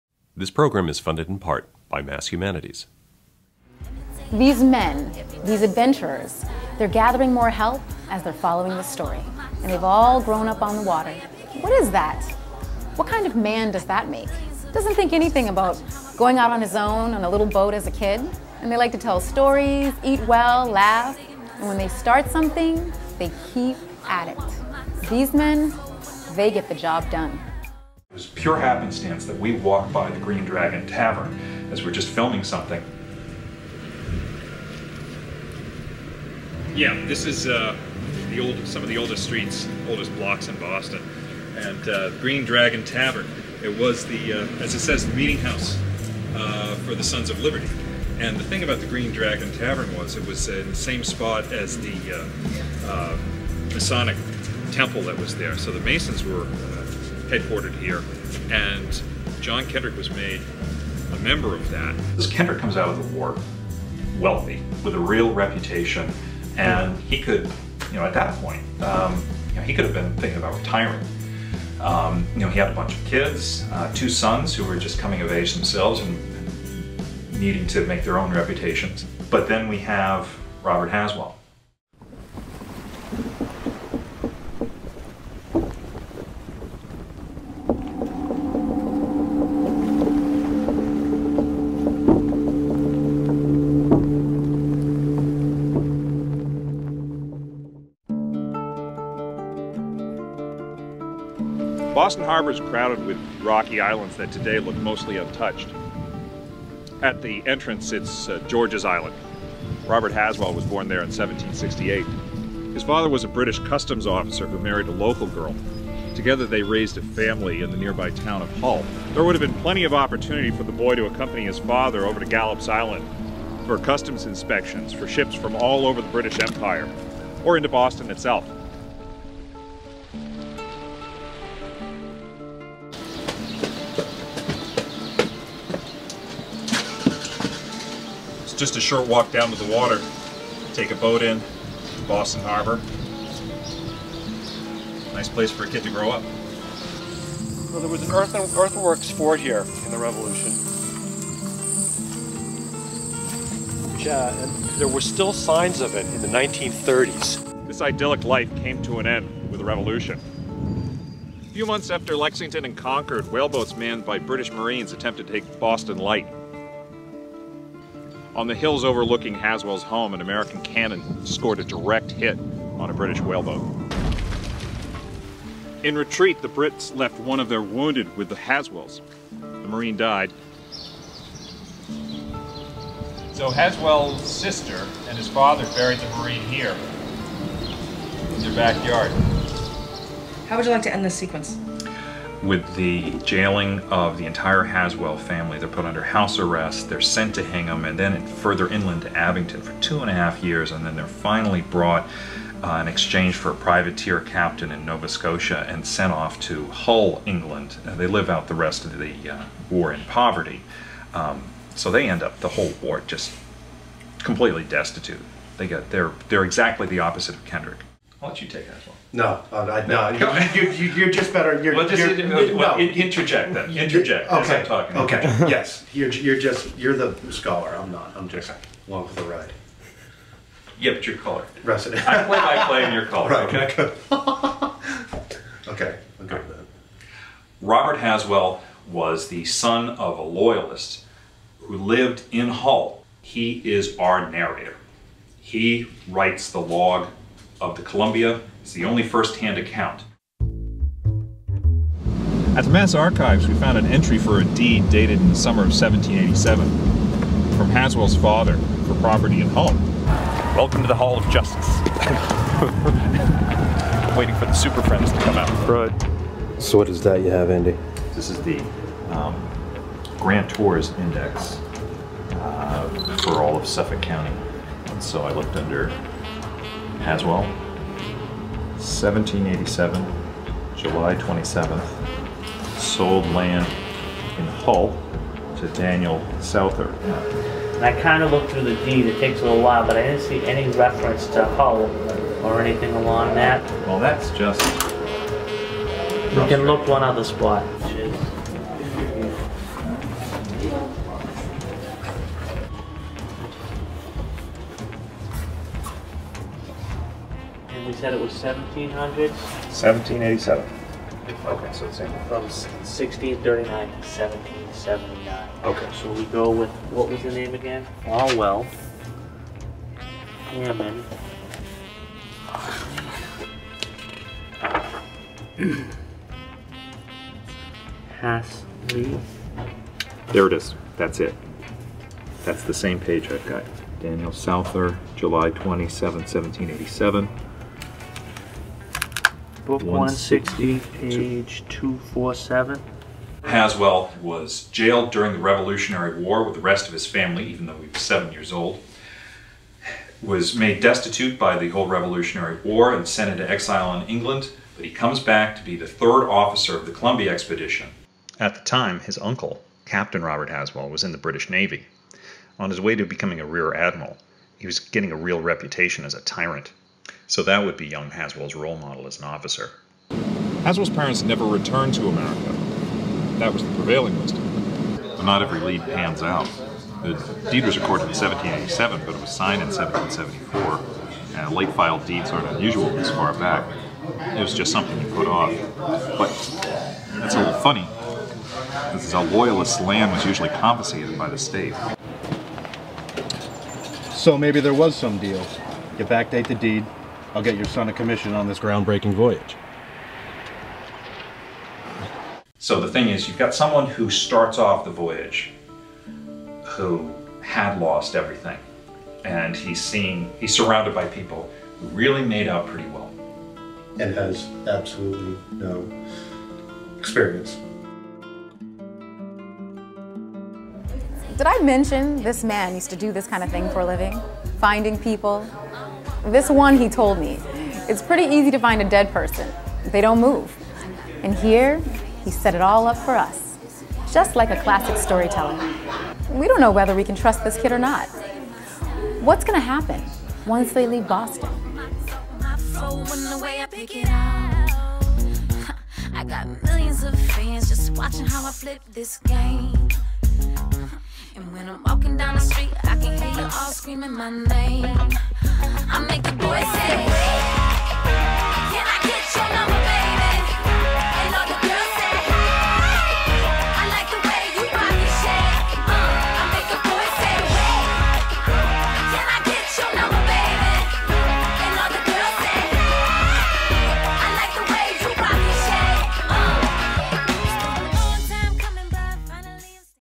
The Loyalist | Columbia Expedition #01.3
"He's the exact opposite of Kendrick." The 19 year-old Third Officer of the ship Columbia. A prisoner of war and refugee before he was ten, Robert Haswell was the son of a British Officer and Loyalist. HRH starts with his birth in Boston Harbor and wartime experiences during the American Revolution. Author of the log of the first Columbia Expedition, he?s maybe not the most reliable narrator.

Locations: Boston, Hull and Chatham, Massachusetts.
Interviews: Don Ritz